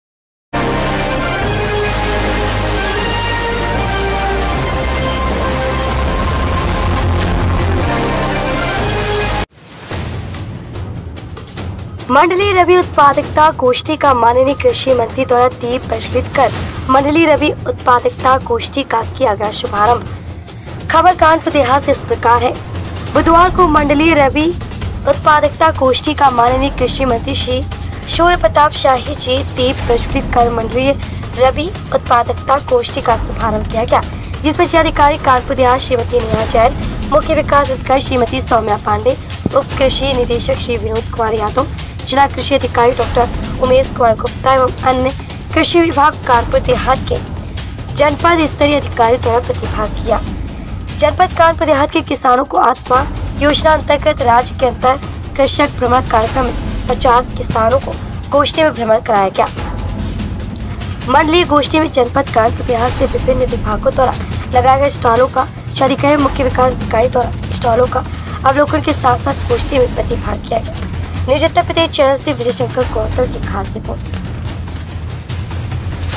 मंडलीय रबी उत्पादकता गोष्ठी का माननीय कृषि मंत्री द्वारा दीप प्रज्जवलित कर गोष्ठी किया गया शुभारंभ
मंडलीय रबी उत्पादकता गोष्ठी का माननीय  कृषि मंत्री द्वारा दीप प्रज्जवलित कर मंडलीय रबी उत्पादकता  गोष्ठी का किया गया शुभारंभ